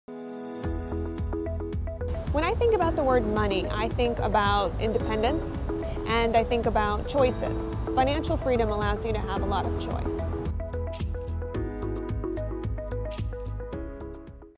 What can money really buy?
Heidi Heikenfeld discusses what money can really buy. What does money mean to you?